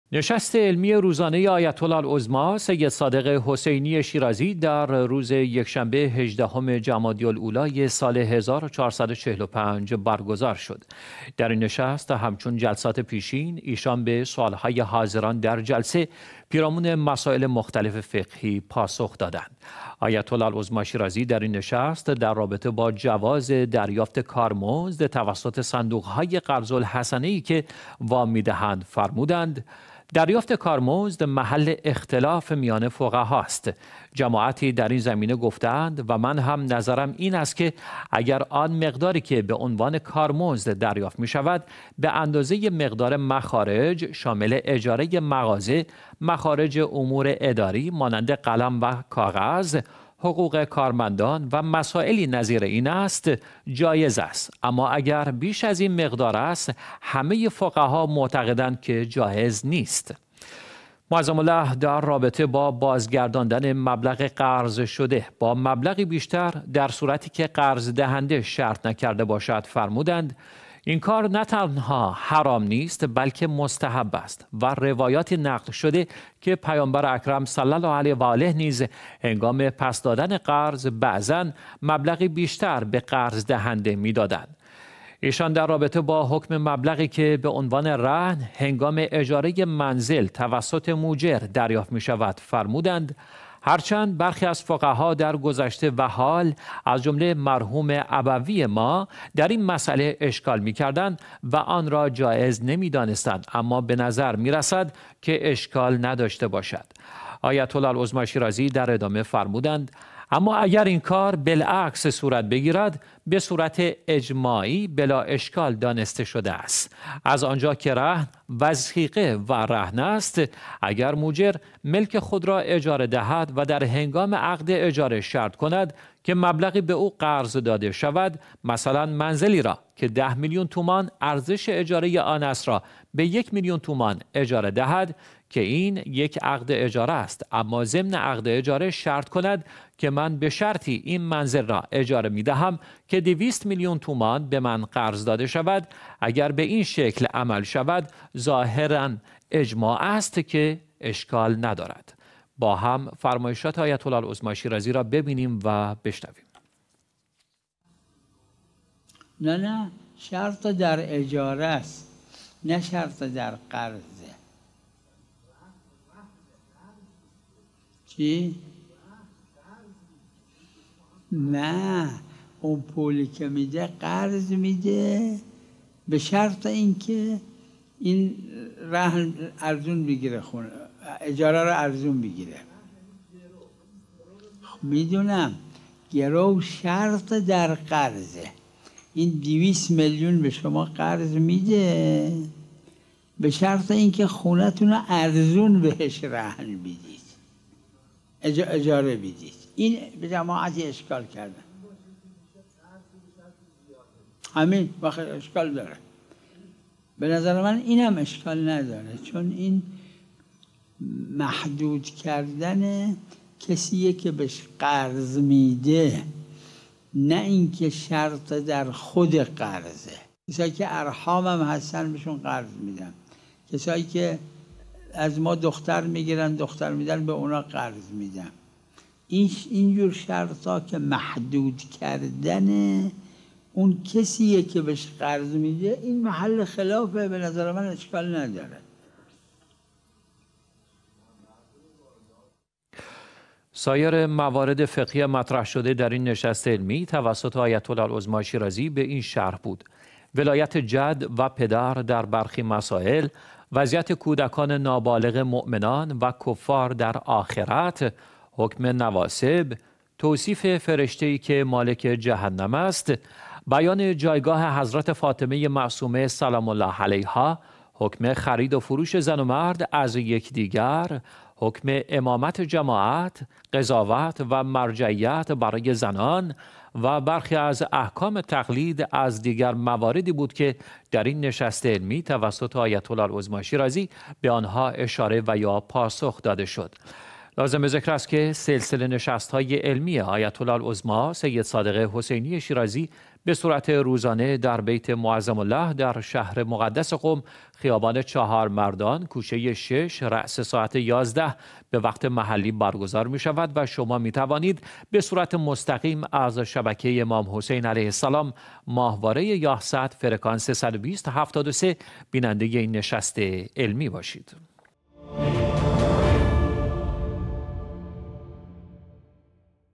آیت الله العظمی شیرازی: اگر موجر شرط کند در قبال اجاره کمتر مبلغی از مستاجر قرض بگیرد بلااشکال است
آیت الله العظمی شیرازی : گرفتن کارمزد به میزان مخارج قرض دهنده ، و گرفتن مبلغ رهن منزل به شکل مرسوم فعلی ، اشکال ندارد ، اما اگر موجر در عقد اجاره شرط کند ، در قبال اجاره ی کمتر ، مبلغی از مستاجر قرض بگیرد ، بنابر اجماع فقها بلااشکال است
نشست علمی روزانه آیت الله العظمی سید صادق حسینی شیرازی ، روز یکشنبه هجدهم جمادی الاول سال ۱۴۴۵ برگزار شد، در این نشست هم چون جلسات پیشین ، ایشان به سوال های حاضران در جلسه ، پیرامون مسائل مختلف فقهی پاسخ دادند.

آیت الله العظمی شیرازی در این نشست فرمودند : دریافت کارمزد ، محل اختلاف میان فقهاست ، جماعتی در این زمینه گفته اند و من هم نظرم این است که اگر آن مقداری که به عنوان کارمزد دریافت می شود ، به اندازه ی مقدار مخارج ، شامل اجاره ی مغازه ، مخارج امور اداری مانند قلم و کاغذ ، حقوق کارمندان ، و مسائلی نظیر این است ، جائز است ، اما اگر بیش از این مقدار است ، همه ی فقها معتقدند که جائز نیست.

معظم له در رابطه با جواز دریافت کارمزد توسط صندوق های قرض الحسنه ای که وام می دهند ، فرمودند : صندوق های قرض الحسنه ای هم که در قبال اعطای وام ، درصدی به عنوان کارمزد دریافت می کنند ، اگر مقدار پول دریافت شده به عنوان کارمزد ، به اندازه ی مخارج صندوق قرض الحسنه است ، اشکال ندارد ، اما اگر بیش از آن مقدار است ، جائز نیست و عدم جواز آن در این صورت ، اجماعی است.

ایشان در ادامه فرمودند : اگر صندوق قرض الحسنه ای محاسبه می کند که سالیانه مثلا پنجاه میلیون تومان ، هزینه ی مخارج آن مانند آب ، برق ، تلفن ، گاز ، اجاره ی محل ، کارمندان و ... می شود ، و متناسب با مخارج و وام اعطا شده ، از برخی افراد که به آن ها وام اعطا می شود ، نیم درصد ، یک درصد ، دو در صد ، سه در صد و ... ، گرفته می شود ، اشکال ندارد ، یعنی فقهایی که این نوع دریافت پول را جائز می دانند ، در شرایطی قائل به این جواز هستند که در قبال چیزی که به آن عرفا کارمزد گفته می شود یعنی مزد کارهایی که انجام می دهند ، اگر به این مقدار پول دریافت می شود ، اشکال نداد ، اما اگر بیش از آن باشد ، محل اشکال است ، و جائز نیست.

آیت الله العظمی شیرازی در رابطه با بازگرداندن مبلغ قرض شده با مبلغی بیشتر ، در صورتی که قرض دهنده شرط نکرده باشد ، فرمودند : این کار نه تنها حرام نیست ، بلکه مستحب است ، و روایاتی نقل شده که پیامبر اکرم صلی الله علیه و آله نیز ، هنگام پس دادن قرض ، بعضا مبلغی بیشتر به قرض دهنده می دادند.

معظم له در ادامه فرمودند : روایتی از پیامبر اکرم صلی الله علیه و آله نقل شده که احتمالا در کتاب اصول کافی هم وجود داشته باشد ، که شخصی از پیامبر اکرم درخواست پول نمود ، و پیامبر در آن زمان پول نداشتند ، لذا از اصحاب پرسیدند که چه کسی به من قرض می دهد ؟ ، که یک نفر از اصحاب گفت من قرض می دهم ، و پیامبر ، پول را به شخص درخواست کننده دادند ، پس از مدتی آن شخصی که به پیامبر پول داده بود ، یکی دو مرتبه پول خود را مطالبه نمود ، اما پیامبر پولی برای ادای قرض نداشتند ، دوباره از اصحاب پرسیدند چه کسی می تواند به من قرض بدهد ؟ ، که یک نفر دیگر از اصحاب به حضرت قرض داد ، و نقل است که دو برابر آن مقدار به او پس دادند ، وقتی آن شخص پرسید که من نیمی از این مبلغ را به شما قرض دادم ، حضرت فرمودند : اشکالی ندارد ، لذا اگر قرض گیرنده ، مبلغی بیشتر از مبلغ قرض ، بازپس بدهد ، نه تنها اشکال ندارد بلکه مستحب است ، اما اگر شرط کند ولو شرط ارتکازی باشد و حتی مکتوب نشده یا بر زبان جاری نشده باشد ، شامل قاعده ی كل قرض جر نفعاً فهو ربا می شود و جائز نیست.

ایشان در رابطه با حکم مبلغی که به عنوان رهن ، هنگام اجاره ی منزل توسط موجر دریافت می شود ، فرمودند : هر چند برخی از فقها در گذشته و حال ، از جمله مرحوم ابوی ما ، در این مساله اشکال می کردند ، و آن را جائز نمی دانستند ، اما به نظر می رسد که اشکال نداشته باشد.

آیت الله العظمی شیرازی در ادامه فرمودند : اما اگر این کار ، بالعکس صورت بگیرد ، به صورت اجماعی بلااشکال دانسته شده است ، از آنجا که رهن ، وثیقه و رهن است ، اگر موجر ، ملک خود را اجاره دهد ، و در هنگام عقد اجاره ، شرط کند که مبلغی به او قرض داده شود ، مثلا منزلی را که ده میلیون تومان ، ارزش اجاره ی آن است را به یک میلیون تومان ، اجاره دهد ، که این یک عقد اجاره است ، اما ضمن عقد اجاره ، شرط کند که من به شرطی این منزل را اجاره می دهم که دویست میلیون تومان به من قرض داده شود ، اگر به این شکل ، عمل شود ، ظاهرا اجماع است که اشکال ندارد.

معظم له تاکید فرمودند : از آنجا که در این صورت ، شرط در اجاره است ، و نه شرط در قرض ، یا به تعبیر دیگر محدود کردن قرض است ، اما به نظر من ، آن چه که امروزه هم به عنوان رهن ، متعارف است ، اشکال ندارد ،

ایشان در ادامه در رابطه با برخی از احکام نماز جماعت فرمودند : اگر امام جماعت ، نمازش برای خودش صحیح است ، ولو بر خلاف نظر ماموم باشد ، نماز جماعت ماموم باطل نیست ، فقط دو استثنا دارد ، اول صلوة جالس بالقائم ، یعنی اگر امام جماعت نمازش نشسته است ، و ماموم ایستاده نماز می خواند ، این از خصائص پیغمبر اکرم صلی الله علیه و آله است که نماز صحیح نیست ، یعنی اگر امام جماعت معذور است و نشسته نماز می خواند ، کسی که نمازش ایستاده است ، نمی تواند قصد جماعت کند ، و پشت سر او نماز بخواند.